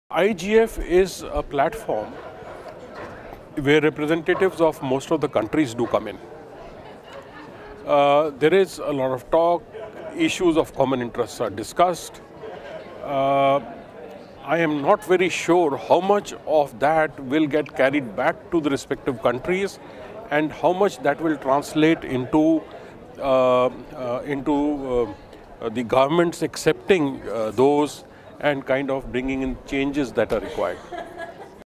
Global IGF 2017: Roopinber Perhar on Multistakeholder Model
Roopinber Perhar, executive council of the Asia Pacific Networking Information Center, shares their answer to the question, “Do multistakeholder efforts like IGF have any impact on the processes they are created to positively influence?” Recorded at the Global Internet Governance Forum 2017 in Geneva, Switzerland, by the Imagining the Internet Center, an initiative of Elon University, North Carolina, USA.